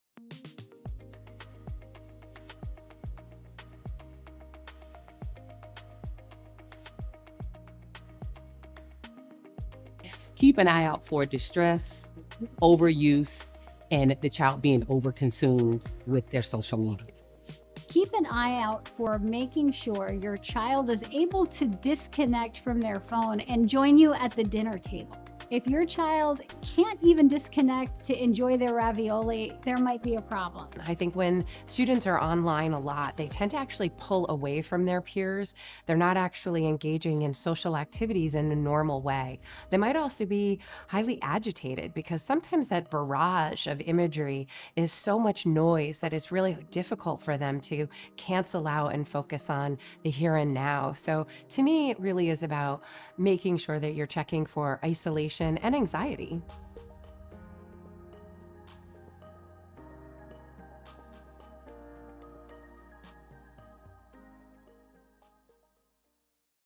How to Tell If Your Child is Spending Too Much Time Online
In the digital age, it’s difficult for kids and parents to put our devices down because they’re designed to be addictive. But how much screen time is too much? Parents, need more questions answered?